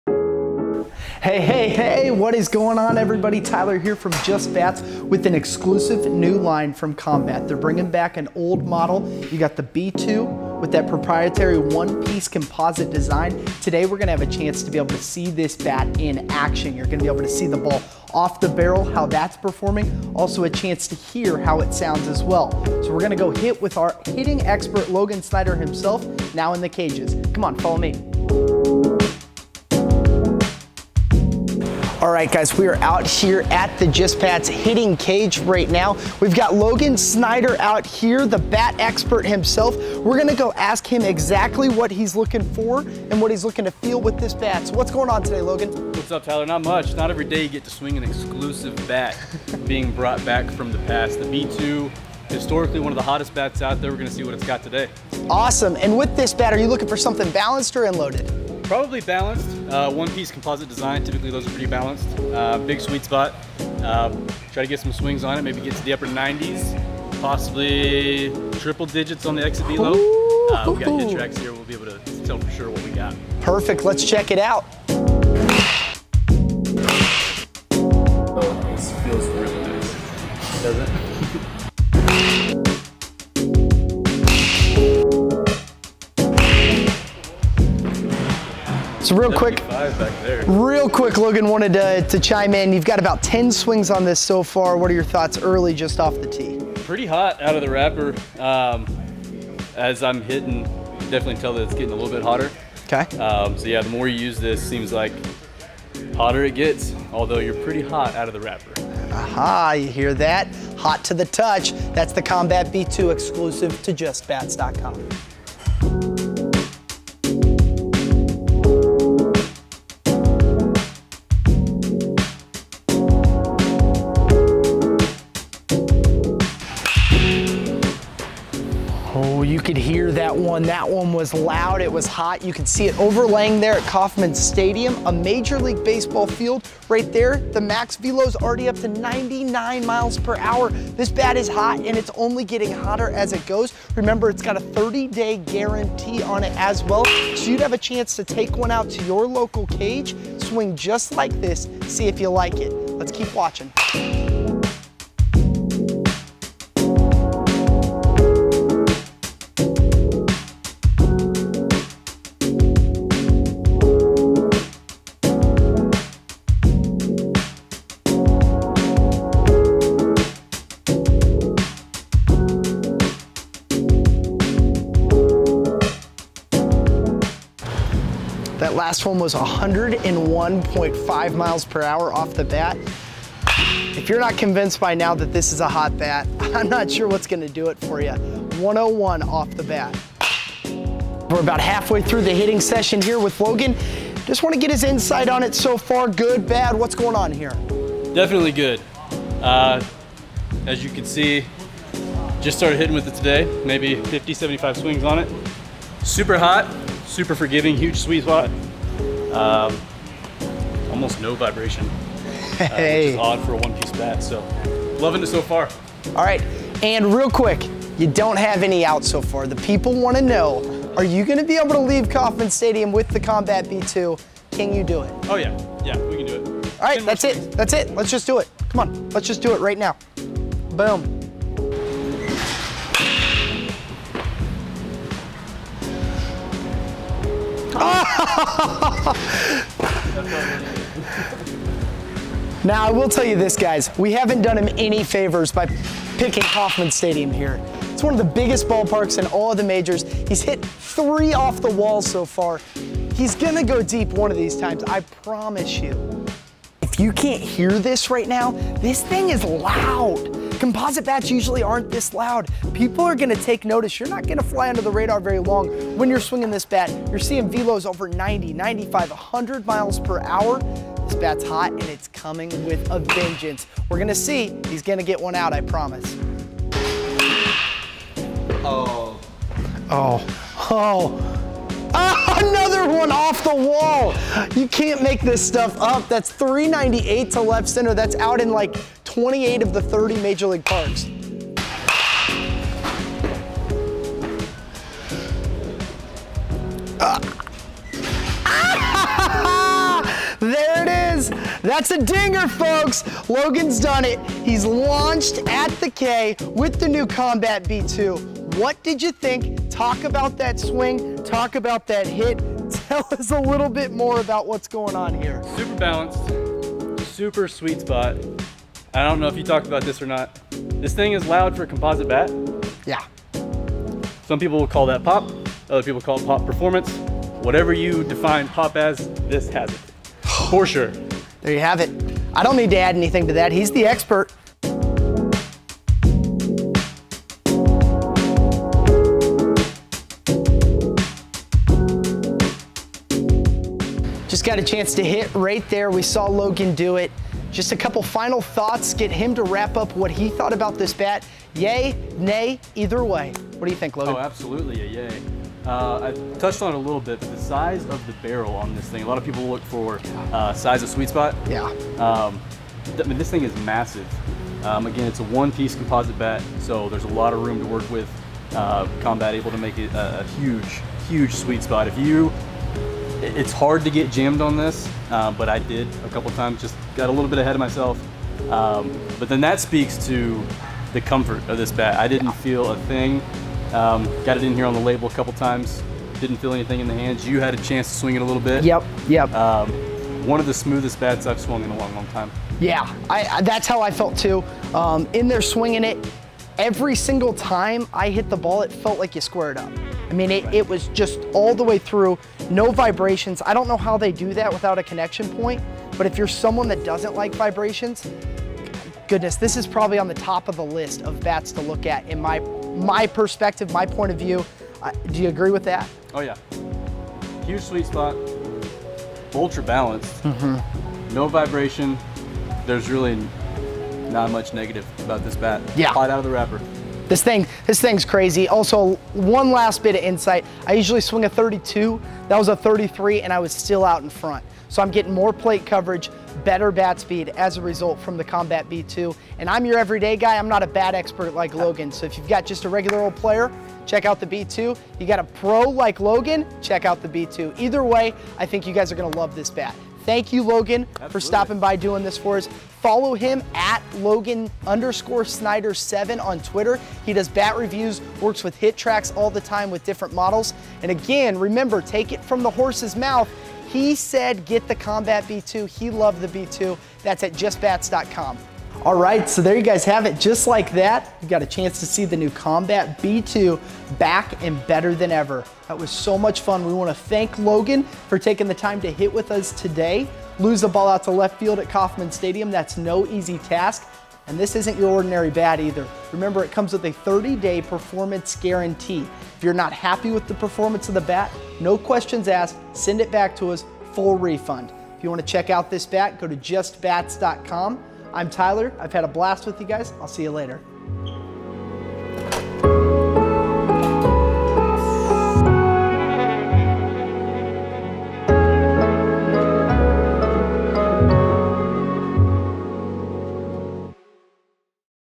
First Look: Brand New Combat B2 Review
Take a look as Logan hits the cages with a brand-new, Combat B2 BBCOR Bat! This JustBats' exclusive model will sport an innovative ATB barrel designed for both power & comfort. Can Logan break the 100mph threshold with Combat's newest composite bat? Will he be able to hit a home run at one of the largest stadiums in the MLB? Let's find out now!